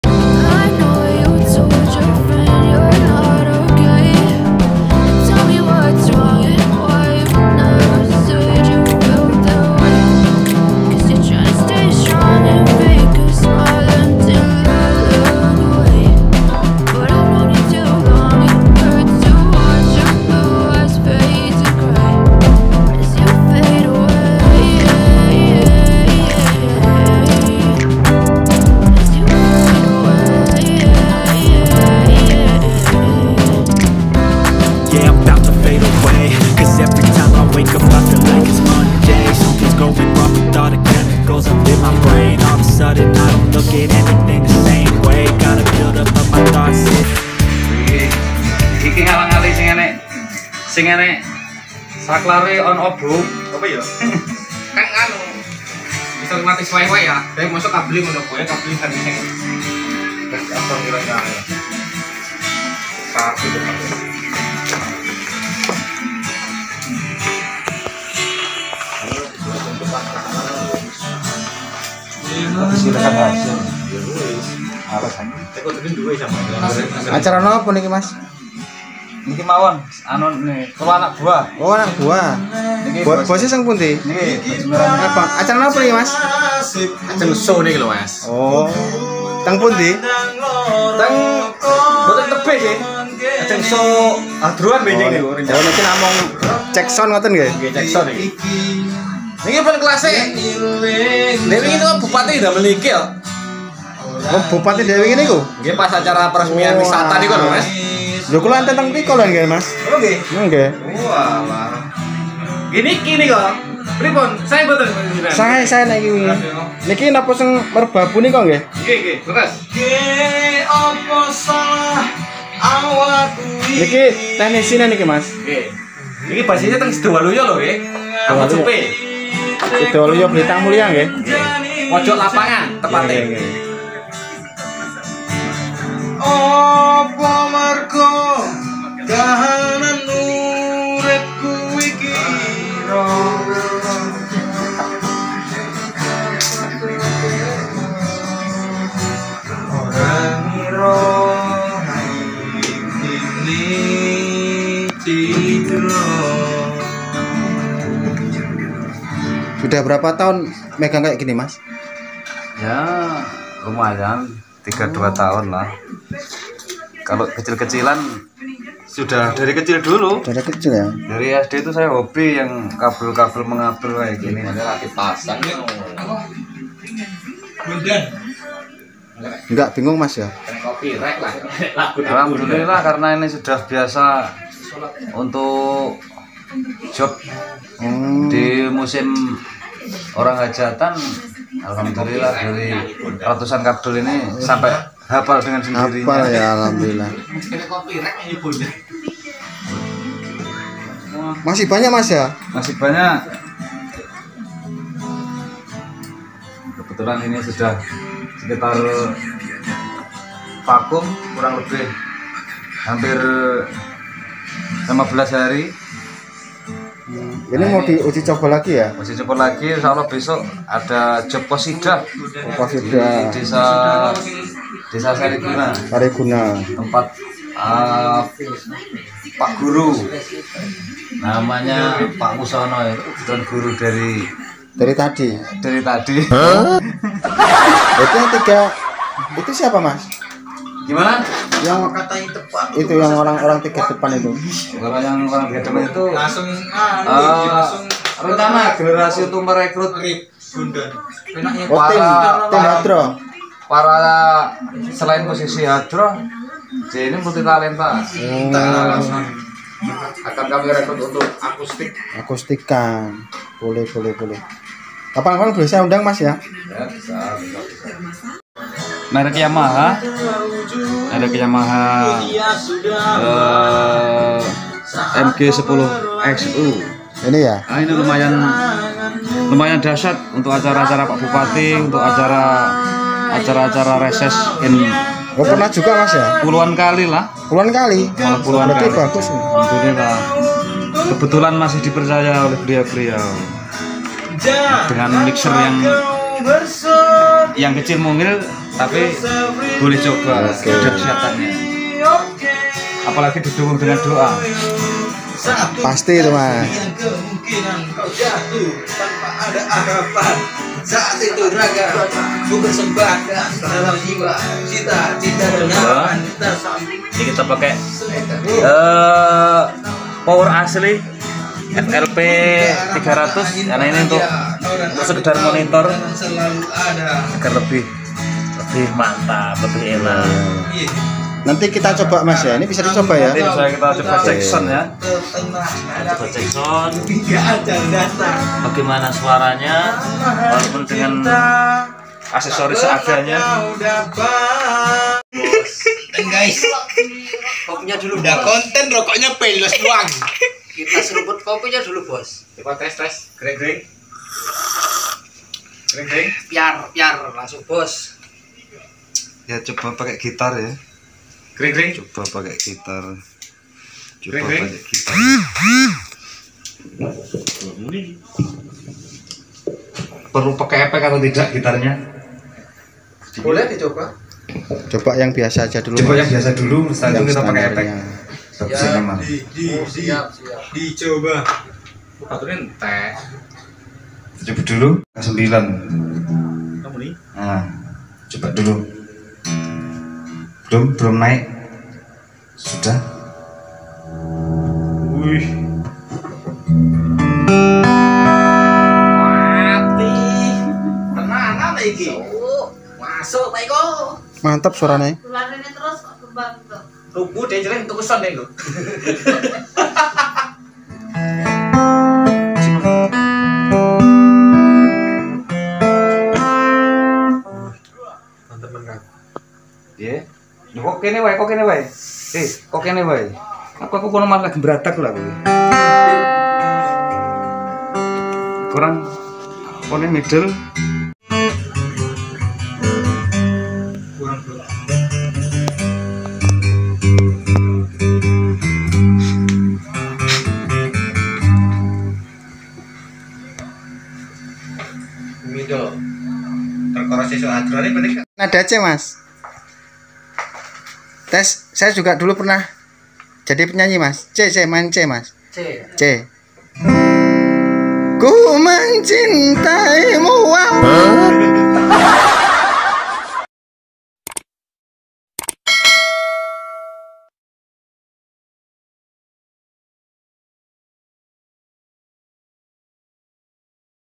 CEK SOUND BERSAMA MERBABU MUSIC || test Gitar Akustik Elektrik
Cek sound bersama "Merbabu Music" Desa Sidowaluyo Kec. Belitang Mulya Kab. OKU Timur, suara sound boleh di coba